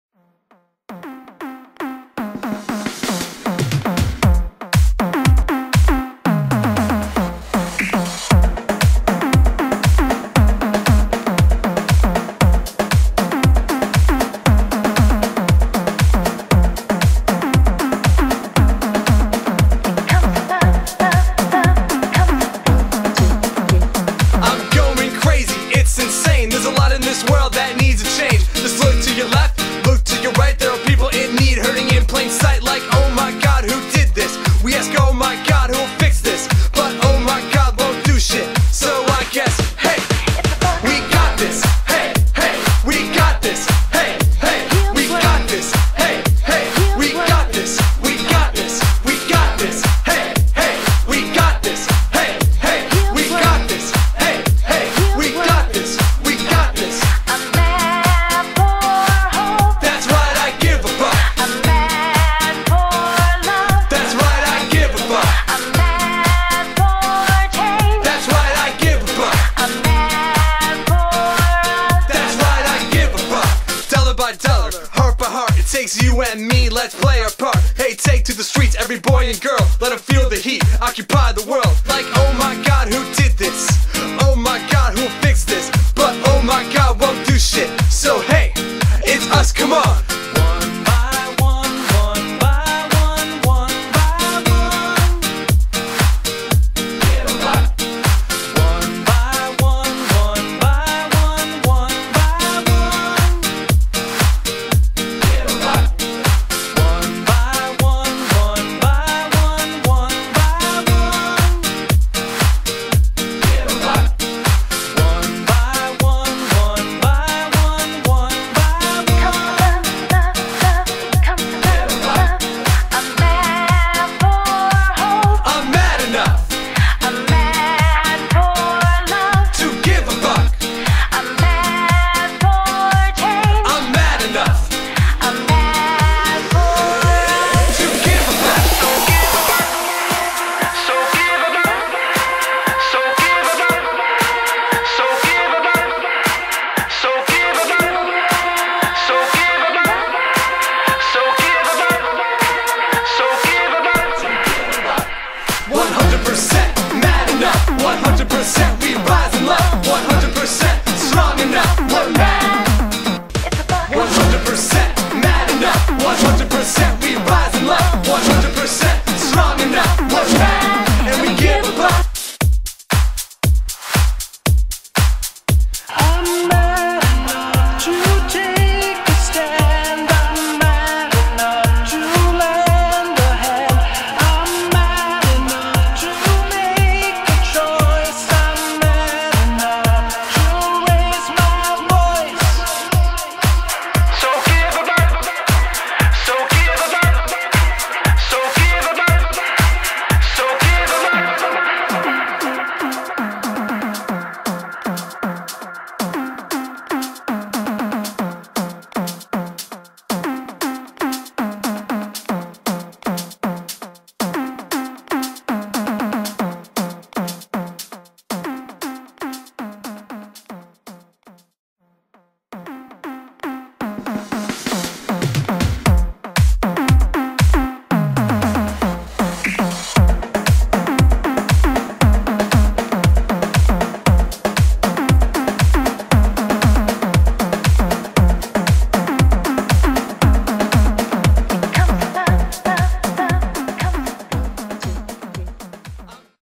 100% MAD 2014 Activities Since Sept Launch
A recap of some of 100% MAD's activities since the September 2014 launch.